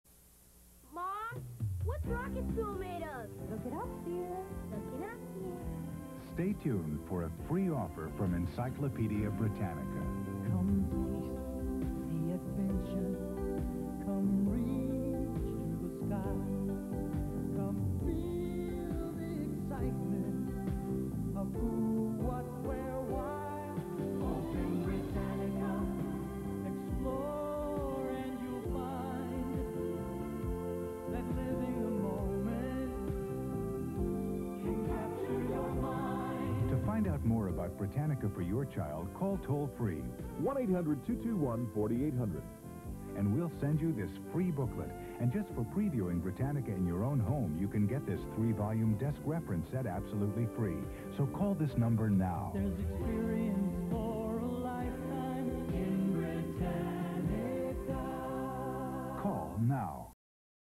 Britannica encylopedia commercial w/ boy, fish & shell & rocket fuel is everclear & hawaiian punch.
a vintage bit of knowledge from the boy in the specs and a damp desk too!!